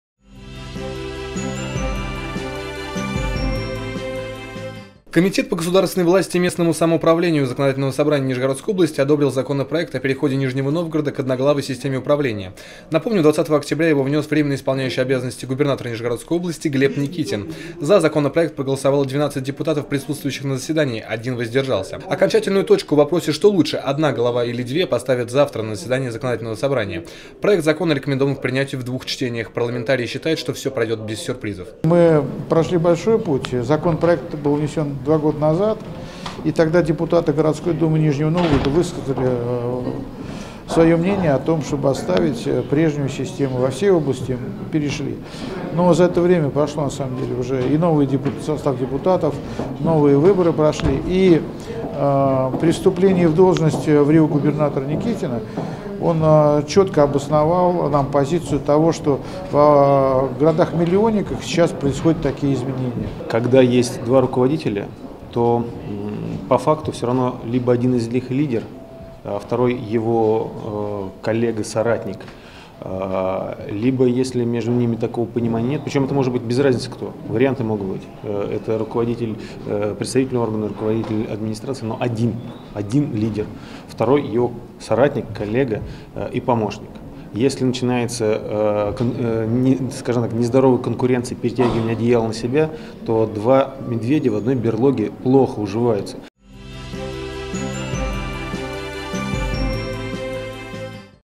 Депутаты ЗСНО одобрили законопроект об одноглавой системе управления Нижним Новгородом
Комитет по государственной власти и местному самоуправлению Законодательного собрания одобрил законопроект о переходе Нижнего Новгорода к одноглавой системе управления. Напомним, что 20 октября его внес временно исполняющий обязанности губернатора Нижегородской области Глеб Никитин. За законопроект проголосовали 12 депутатов, один воздержался. Окончательную точку в вопросе об одноглавой системе поставят 26 октября на заседании Законодательного собрания. Проект закона рекомендован к принятию в двух чтениях. Парламентарии считают, что все пройдет без сюрпризов. Подробности — в сюжете «Столицы Нижний».